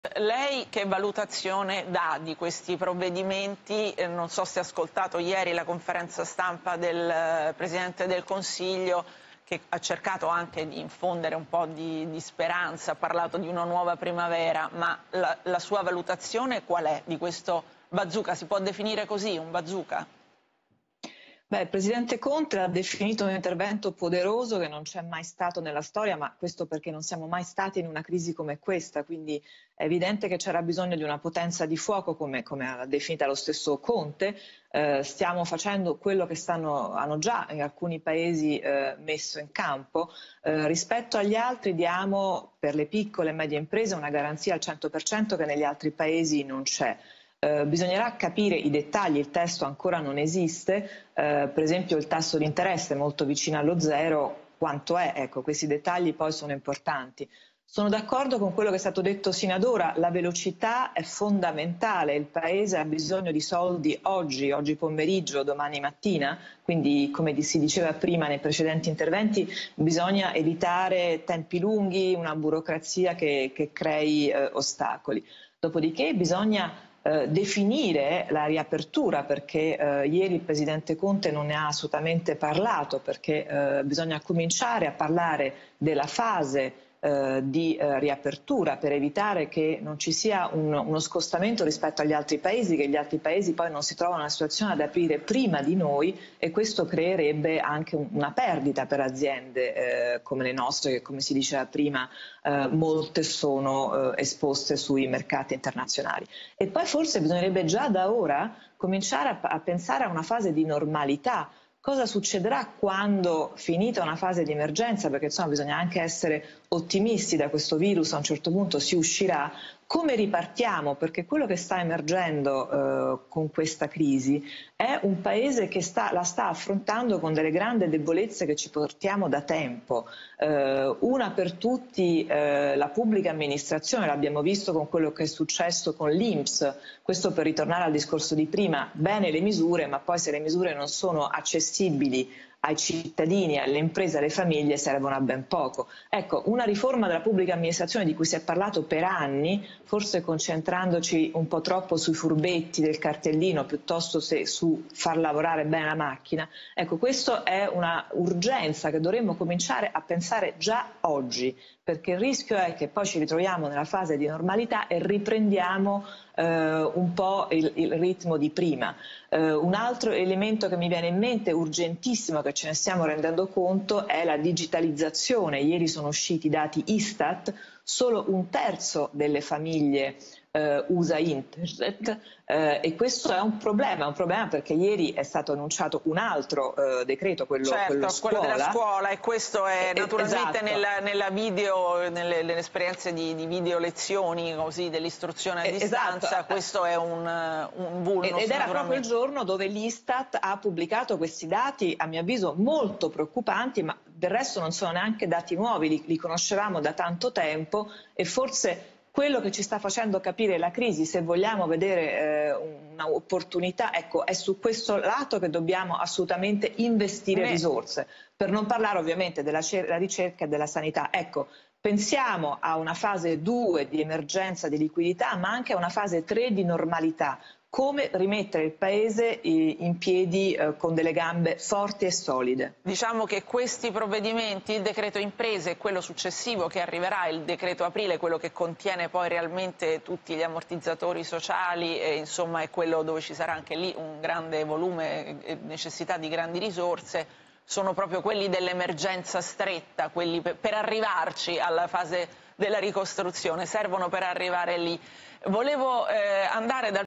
400 miliardi alle imprese, il commento di Veronica De Romanis: "La velocità è fondamentale"
Il commento dell'economista Veronica De Romanis sulle misure economiche a sostegno delle imprese varate dal governo: "Cosa succederà quando sarà finita l'emergenza?"